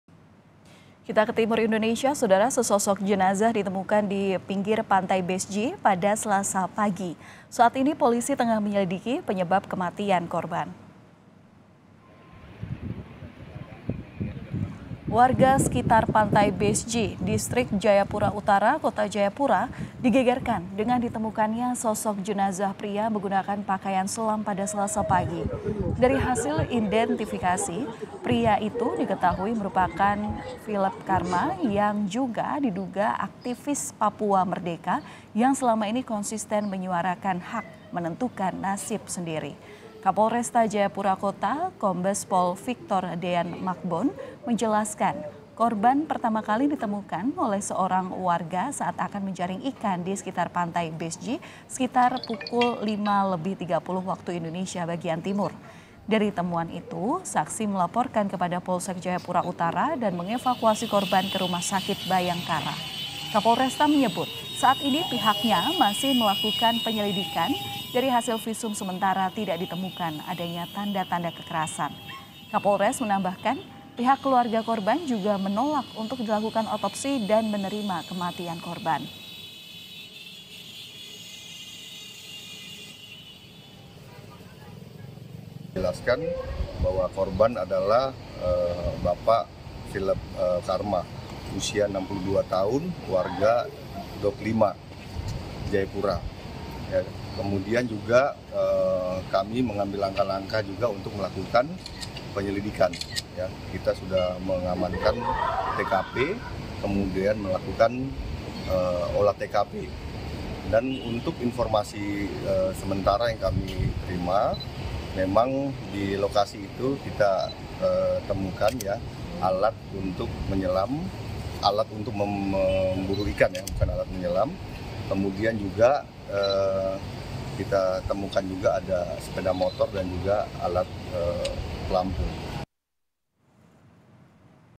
Jenazah Aktivis Papua Merdeka Filep Karma Ditemukan di Tepi Pantai, Diduga Tenggelam
KOMPAS.TV - Sesosok jenazah ditemukan di pinggir pantai "Base G" pada Selasa pagi 1 November 2022.

Saat ini polisi tengah menyelidiki penyebab kematian korban.

Warga sekitar pantai "Base G" Distrik Jayapura Utara Kota Jayapura digegerkan dengan ditemukannya sosok jenazah pria menggunakan pakaian selam pada Selasa pagi1 November 2022.

Dari hasil identifikasi, pria itu diketahui merupakan Filep Karma yang juga diduga Aktivis Papua Merdeka dan selama ini konsisten menyuarakan hak menentukan nasib sendiri.

Kapolresta Jayapura Kota, Kombes Pol Vicktor Dean Mackbon menjelaskan korban pertama kali ditemukan oleh seorang warga saat akan menjaring ikan di sekitar Pantai Base G sekitar 05.30 WIT.

Dari temuan itu, saksi melaporkan kepada Polsek Jayapura Utara dan mengevakuasi korban ke Rumah Sakit Bayangkara.

Kapolresta menyebut, saat ini pihaknya masih melakukan penyelidikan dari hasil visum sementara tidak ditemukan adanya tanda-tanda kekerasan.

Kapolres menambahkan pihak keluarga korban juga  menolak untuk dilakukan otopsi dan menerima kematian korban.